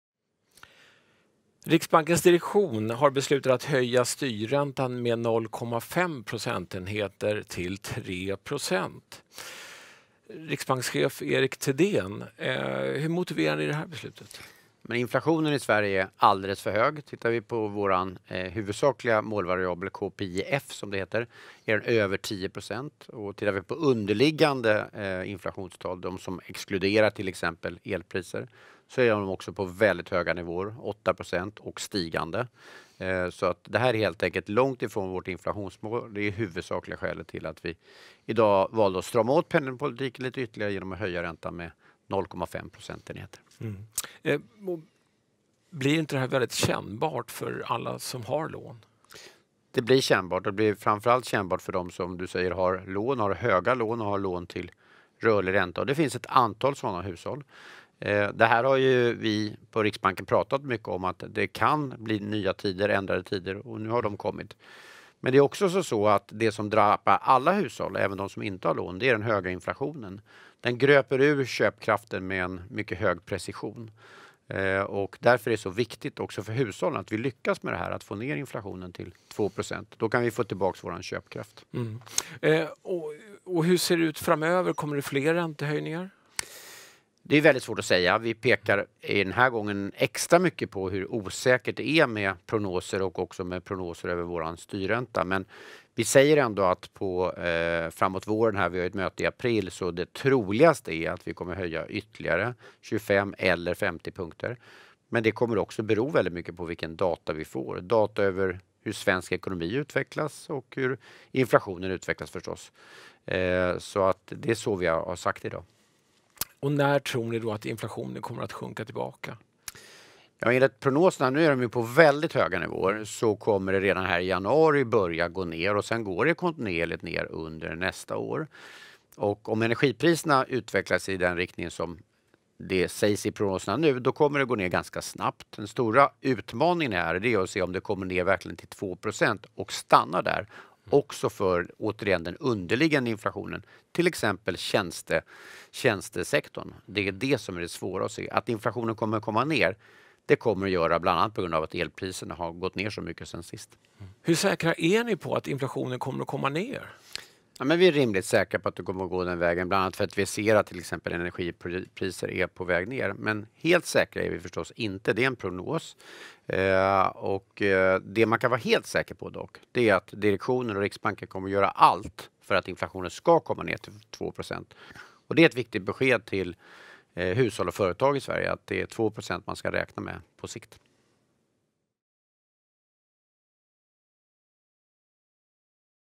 Erik Thedéen intervjuas om det penningpolitiska beslutet i februari 2023
Riksbanken höjer styrräntan med 0,5 procentenheter till 3,0 procent. I den här videon förklarar riksbankschef Erik Thedéen varför.